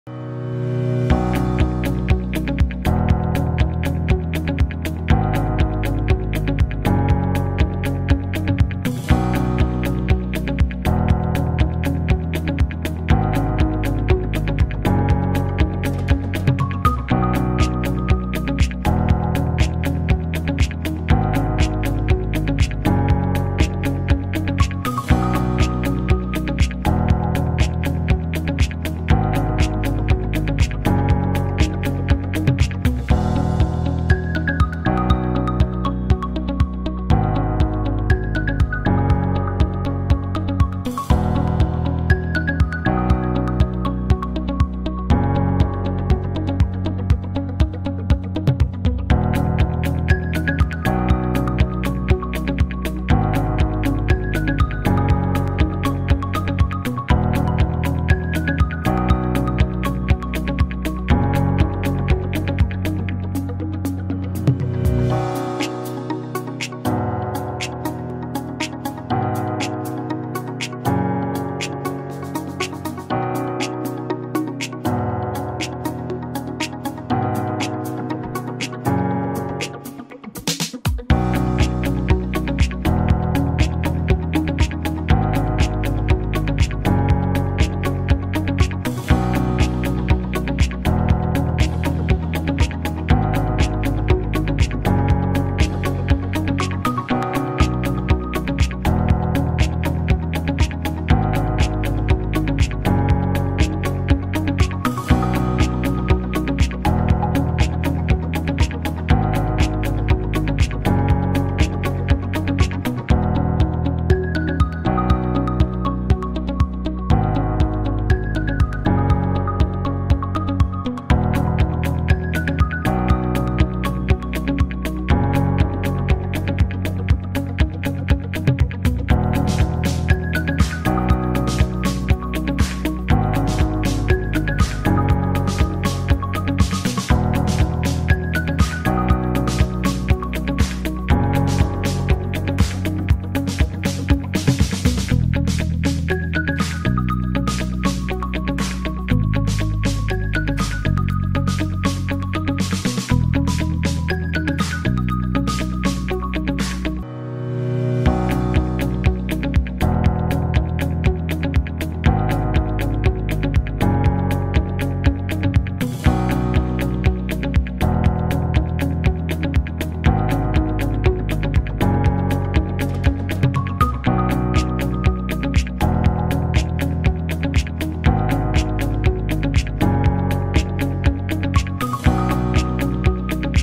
Sieraden maken: ImpressArt afwerking-en polijstblokken ♡ DIY
Gebruik het matte ImpressArt afwerking polijstblok om metalen van een zachte, niet glanzende afwerking te voorzien. Voor gebruik op alle ImpressArt stamp labels! Gebruik de zijde met grove korrels om scherpe randen te verzachten en zachte metalen te vormen. Gebruik de zijde met de medium korrel om een mat geborsteld oppervlak te creëren.

Gebruik het ImpressArt hoogglans afwerkingsblok polijst om metalen een briljante, hoogglanzende afwerking te geven. Gebruik de zijde met fijne korrels om stamp labels snel te reinigen en te polijsten. Gebruik de zijde met de extra fijne korrel voor het polijsten tot een hoge glans zoals te zien in deze DIY!